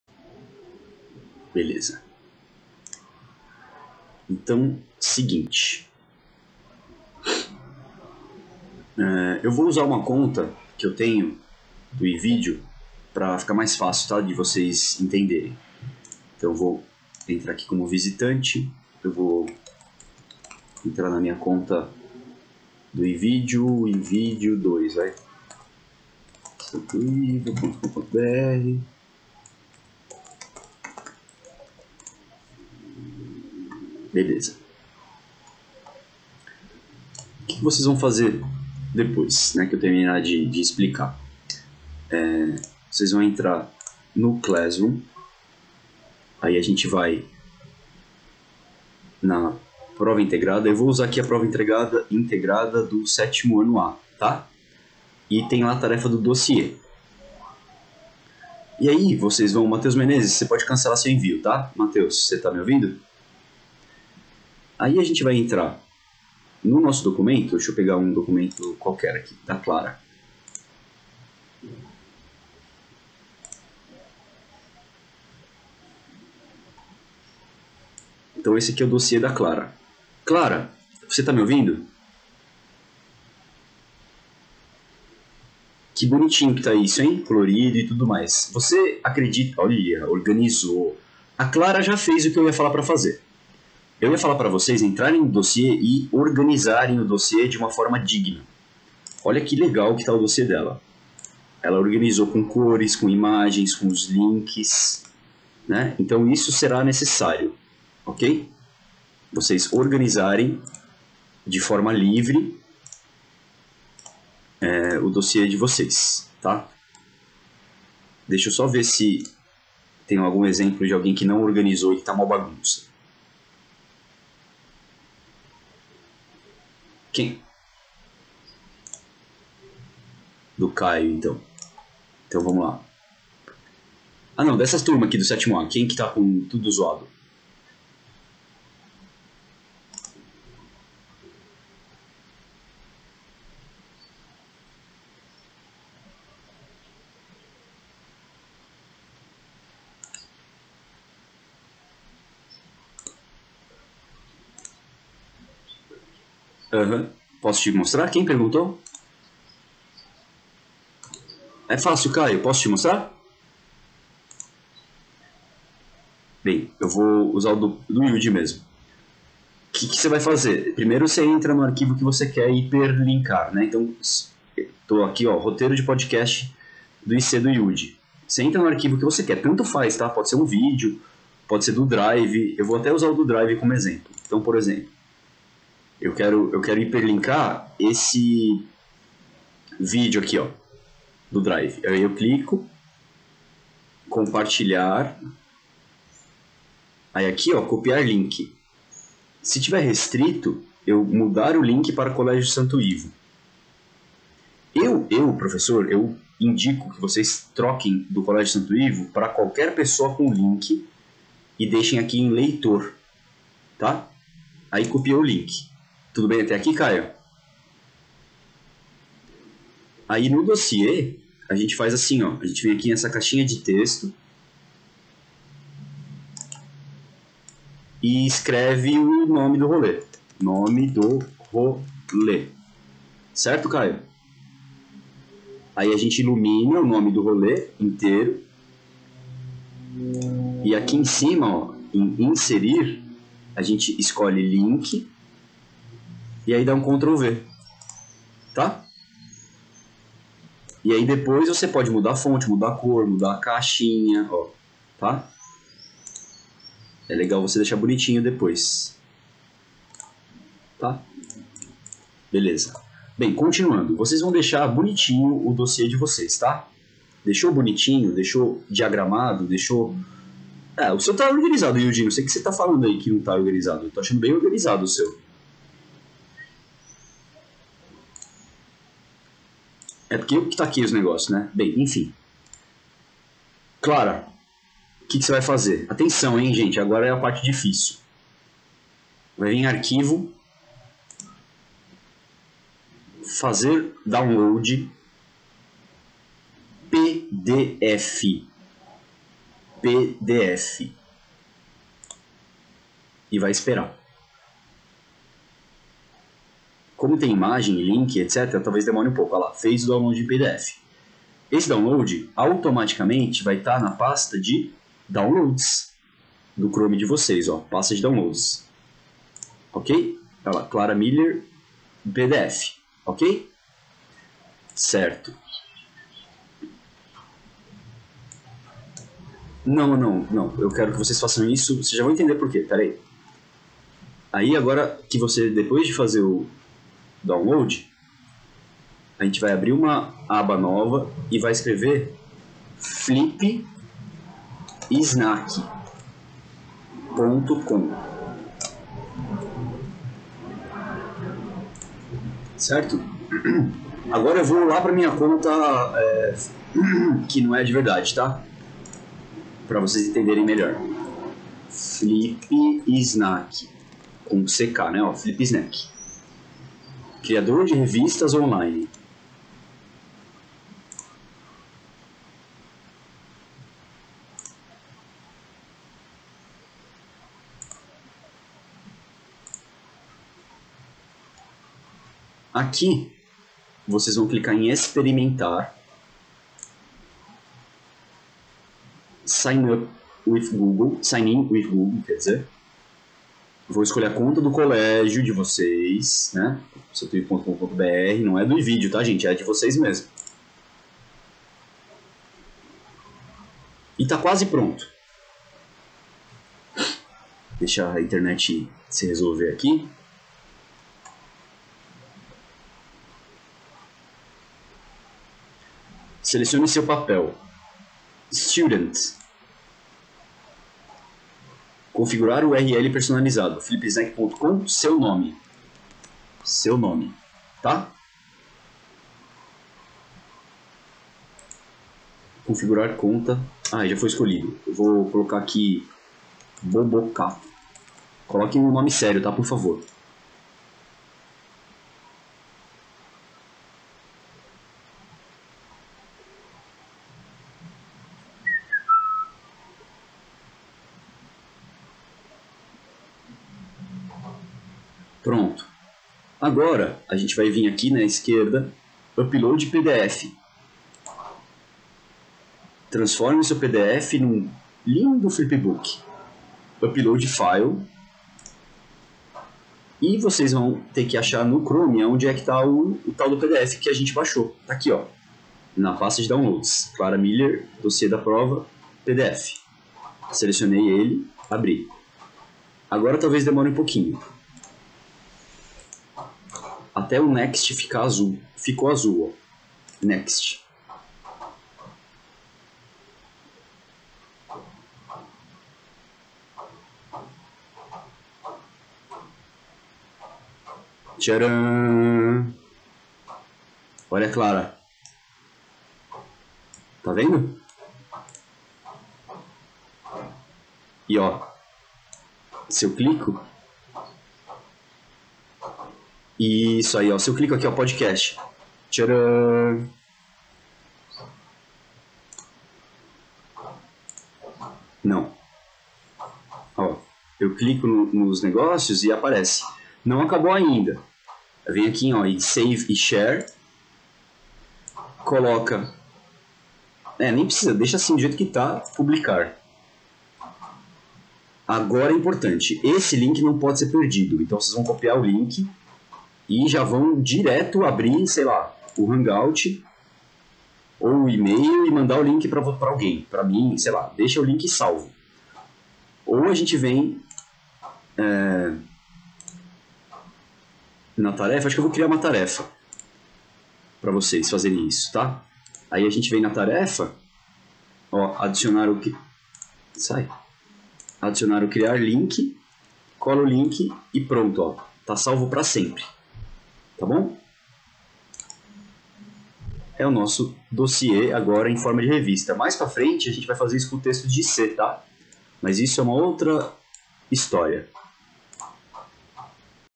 DOSSIE DIGITALIZADO
Faça o download de seu DOSSIÊ (Arquivo, FAZER DOWNLOAD COMO PDF)
Acesse o FLIPSNACK.COM, crie uma conta experimental e faça o UPLOAD do arquivo baixado em PDF.
Dê um nome para sua conta e arquivo
Publique
Copie o link e cole na TAREFA ONDE ESTÁ LOCALIZADO O DOSSIÊ OFICIAL.